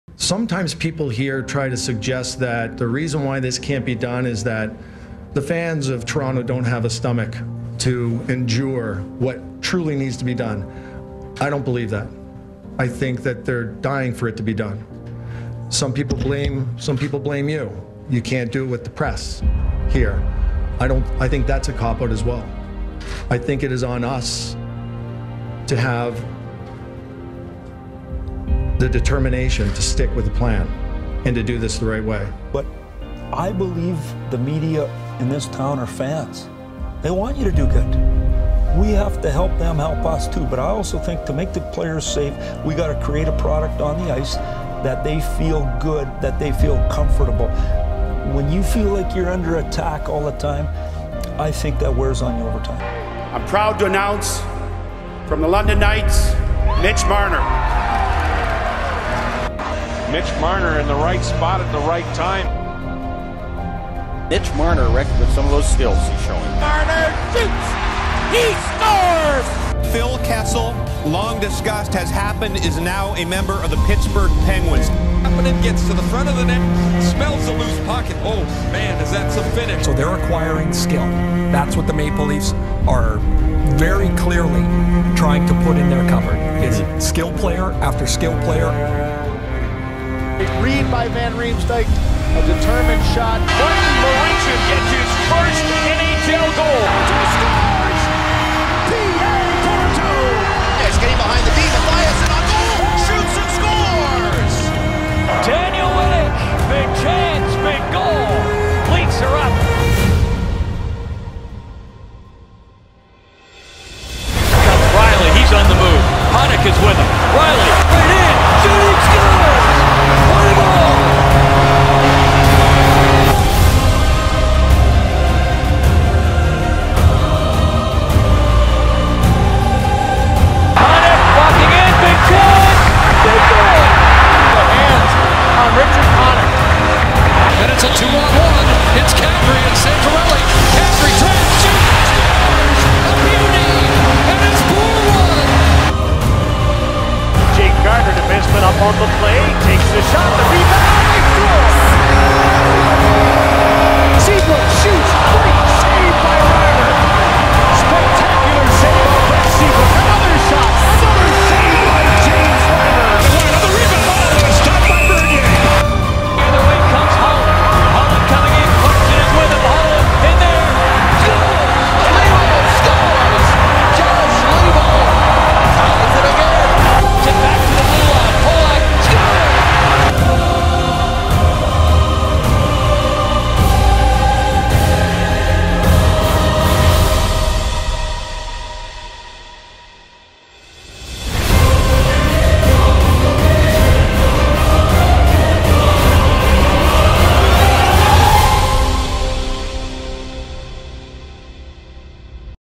l TORONTO MAPLE LEAFS l TURNING THE PAGE l 2015-16 l HD l
After a disappointing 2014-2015 season for the team, eyes turn toward Shanny and co. as they try to steer us back on course. CANT WAIT!